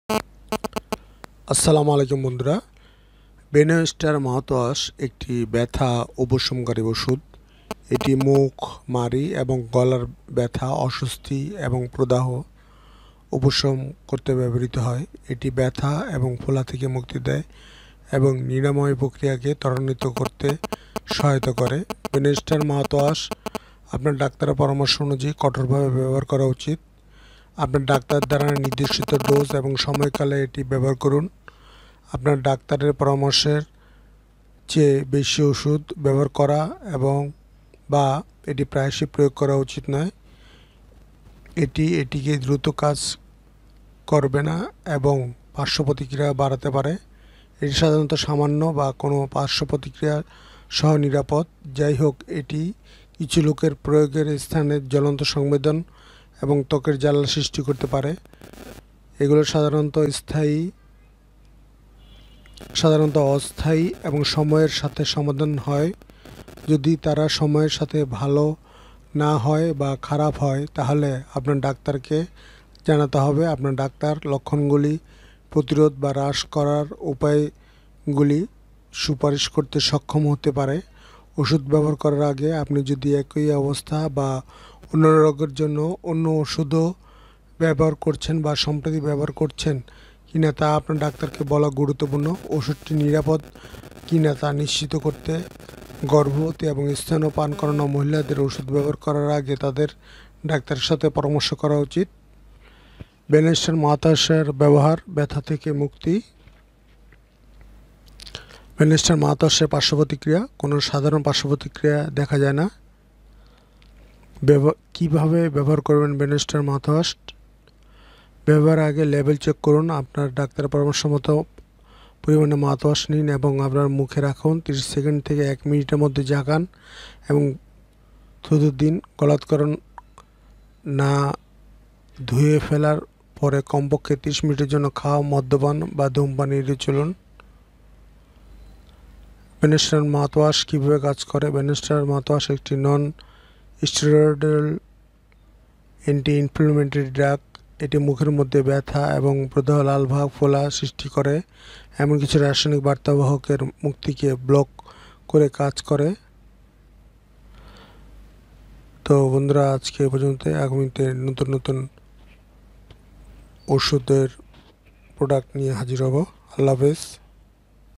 Benostar Mouthwash Review
Assalamualaikum.
Benostar Mouthwash একটি ব্যথা উপশমকারী ওষুধ৷ এটি মুখ, মাড়ি এবং গলার ব্যথা, অস্বস্তি এবং প্রদাহ উপশম করতে ব্যবহৃত হয়। এটি ব্যথা এবং ফোলা থেকে মুক্তি দেয় এবং নিরাময় প্রক্রিয়াকে ত্বরান্বিত করতে সহায়তা করে। Benostar Mouthwash আপনার ডাক্তারের পরামর্শ অনুযায়ী কঠোরভাবে ব্যবহার করা উচিত। আপনার ডাক্তার দ্বারা নির্দেশিত ডোজ এবং সময়কালে এটি ব্যবহার করুন। আপনার ডাক্তারের পরামর্শের চেয়ে বেশি ওষুধ ব্যবহার করা বা এটি প্রায়শই প্রয়োগ করা উচিত নয়। এটি এটিকে দ্রুত কাজ করবে না এবং পার্শ্ব প্রতিক্রিয়া বাড়াতে পারে। এটি সাধারণত সামান্য বা কোন পার্শ্ব প্রতিক্রিয়া সহ নিরাপদ। যাইহোক, এটি কিছু লোকের প্রয়োগের স্থানে জ্বলন্ত সংবেদন এবং ত্বকের জ্বালা সৃষ্টি করতে পারে। এগুলি সাধারণত অস্থায়ী এবং সময়ের সাথে সমাধান হয়। যদি তারা সময়ের সাথে ভাল না হয় বা খারাপ হয়, তাহলে আপনার ডাক্তারকে জানাতে হবে। আপনার ডাক্তার লক্ষণগুলি প্রতিরোধ বা হ্রাস করার উপায়গুলি সুপারিশ করতে সক্ষম হতে পারে। ওষুধ ব্যবহার করার আগে, আপনি যদি একই অবস্থা বা অন্যান্য রোগের জন্য অন্য কোনও ওষুধ ব্যবহার করছেন বা সম্প্রতি ব্যবহার করছেন কিনা তা আপনার ডাক্তারকে বলা গুরুত্বপূর্ণ। ওষুধটি নিরাপদ কিনা তা নিশ্চিত করতে, গর্ভবতী এবং স্তন্যপান করানো মহিলাদের ওষুধ ব্যবহার করার আগে তাদের ডাক্তারের সাথে পরামর্শ করা উচিত।

Benostar Mouthwash এর ব্যবহার

ব্যাথা থেকে মুক্তি
Benostar Mouthwash এর পার্শ্বপ্রতিক্রিয়া

সাধারণ
কোন সাধারণ পার্শ্ব প্রতিক্রিয়া দেখা যায় না
কিভাবে ব্যবহার করবেন Benostar Mouthwash

ব্যবহারের আগে লেবেল চেক করুন। আপনার ডাক্তারের পরামর্শ মতো পরিমাণে মাউথওয়াশ নিন এবং আপনার মুখে রাখুন। 30 সেকেন্ড থেকে 1 মিনিটের জন্য ঝাঁকান এবং থুতু দিন। গলাধ: করণ না. ধুয়ে ফেলার পরে কমপক্ষে 30 মিনিটের জন্য খাওয়া, মদ্যপান বা ধূমপান এড়িয়ে চলুন।

Benostar Mouthwash কিভাবে কাজ করে

Benostar Mouthwash একটি নন-স্টেরয়েডাল অ্যান্টি-ইনফ্লেমেটরি ড্রাগ (NSAID)। এটি মুখের মধ্যে ব্যথা এবং প্রদাহ (লালভাব এবং ফোলা) সৃষ্টি করে এমন কিছু রাসায়নিক বার্তাবাহকের মুক্তিকে ব্লক করে কাজ করে।

আপনি যদি Benostar Mouthwash নিতে ভুলে যান?

আপনি যদি Benostar Mouthwash এর একটি ডোজ মিস করেন, যত তাড়াতাড়ি সম্ভব এটি গ্রহণ করুন। যাইহোক, যদি আপনার পরবর্তী ডোজের প্রায় সময় হয়ে যায়, মিস করা ডোজটি এড়িয়ে যান এবং আপনার নিয়মিত সময়সূচীতে ফিরে যান। ডোজ দ্বিগুণ করবেন না।

quick tips
Quick Tips
Benostar Mouthwash মুখ, মাড়ি এবং গলার ব্যথা এবং প্রদাহ (লালভাব এবং ফোলা) উপশম করতে ব্যবহৃত হয়।
এটি শুধুমাত্র মুখ ধোয়া বা গার্গল করার জন্য বোঝানো হয়। ব্যবহারের পরে ধুয়ে ফেলুন, এটি গিলে ফেলবেন না।
এটা সাধারণত undiluted ব্যবহার করা উচিত. যদি এটি আপনার মুখে দংশন করে তবে আপনি এটি ব্যবহারের আগে সমান পরিমাণ জল দিয়ে পাতলা করতে পারেন।
আপনার চোখে এটি পাওয়া এড়িয়ে চলুন। আপনি যদি ভুলবশত এটি আপনার চোখে পড়ে তবে প্রচুর জল দিয়ে অবিলম্বে ধুয়ে ফেলুন।
এটি সাত দিনের বেশি ব্যবহার করবেন না, যদি না আপনার ডাক্তারের পরামর্শ থাকে।

description
Brief Description
Indication

মুখ ও গলায় ব্যথা বা জ্বালা, তীব্র ফ্যারঞ্জাইটিস, বিকিরণ-সম্পর্কিত মিউকোসাইটিস

Administration

15 মিলি মাউথওয়াশ মুখের চারপাশে ধুয়ে ফেলুন বা গার্গল হিসাবে ব্যবহার করুন। 20 থেকে 30 সেকেন্ড পরে, এটি থুতু আউট. মাউথওয়াশ গিলে ফেলবেন না।

Adult Dose

মুখ/গলা ব্যাথা বা মুখ ও গলার জ্বালা, তীব্র ফ্যারঞ্জাইটিস প্রাপ্তবয়স্ক: ব্যথা কম হওয়া পর্যন্ত প্রতি 1½ থেকে 3 ঘন্টা অন্তর 15 মিলি 0.15% দ্রবণ (যদি দংশন হয় তবে পানি দিয়ে মিশ্রিত) ধুয়ে ফেলুন বা গার্গেল করুন। অথবা 15 মিলি মুখে ধুয়ে ফেলুন (30 সেকেন্ডের জন্য, মুখ থেকে বের করে দেওয়ার পরে) দিনে 3-4 বার রেডিয়েশন থেরাপির 1 দিন আগে চিকিত্সা শুরু করুন এবং চিকিত্সা চলাকালীন প্রতিদিন চালিয়ে যান

Contraindication

Benzydamine Hydrochloride এর যে কোনো উপাদানের প্রতি অতি সংবেদনশীলতার ইতিহাস আছে এমন রোগীদের ক্ষেত্রে নিষেধ।

বেনজিডামাইন হাইড্রোক্লোরাইড প্রধানত অন্যান্য প্রো-ইনফ্ল্যামেটরি সাইটোকাইনগুলিকে প্রভাবিত না করে টিউমার নেক্রোসিস ফ্যাক্টর-আলফা (TNF-α) এবং ইন্টারলেউকিন-1β (IL-1β) এর মতো প্রো-ইনফ্ল্যামেটরি সাইটোকাইনগুলির সংশ্লেষণকে বাধা দিয়ে কাজ করে। তদুপরি, এটি একটি স্থানীয় অবেদনিক প্রভাবে সক্ষম যা পদার্থ পি এর মতো প্রদাহজনক মধ্যস্থতাকারীদের মুক্তিকে বাধা দেওয়ার ক্ষমতার সাথে সম্পর্কিত হতে পারে।

Precaution

আনডিলুটেড বেনজাইডামিন হাইড্রোক্লোরাইডের ব্যবহার মিউকোসাল ত্রুটিযুক্ত রোগীদের জ্বলন্ত সংবেদন দ্বারা উদ্ভাসিত স্থানীয় জ্বালা তৈরি করতে পারে। প্রয়োজনে, এটি হালকা গরম জলে (1:1) পাতলা করা যেতে পারে। যেহেতু এটি মৌখিক শ্লেষ্মা থেকে শোষিত হয় এবং বেশিরভাগই প্রস্রাবে অপরিবর্তিতভাবে নির্গত হয়, তাই রেনাল প্রতিবন্ধকতাযুক্ত রোগীদের ক্ষেত্রে এর পদ্ধতিগত ক্রিয়াকলাপের সম্ভাবনা বিবেচনা করা উচিত।

Side Effect

1-10% জ্বালাপোড়া / হুল ফোটানো সংবেদন, বমি বমি ভাব/বমি, অসাড়তা ফ্রিকোয়েন্সি সংজ্ঞায়িত নয় কাশি, তন্দ্রা, শুষ্ক মুখ, মাথাব্যথা, ফ্যারিঞ্জিয়াল জ্বালা

Pregnancy Category Note

গর্ভাবস্থার বিভাগ: উপলব্ধ নয়। গর্ভবতী মহিলাদের মধ্যে নিরাপত্তা প্রতিষ্ঠিত হয়নি। স্তন্যদান: দুধে নির্গমন অজানা; সতর্কতার সাথে ব্যবহার করুন

অবশ্যই ডাক্তারের পরামর্শ মোতাবেক ব্যবহার করবেন, আমাদের চ্যানেলের ভিডিও দেখে নিজে নিজে ব্যবহার করলে চ্যানেল কর্তৃপক্ষ দায়ী থাকবে না।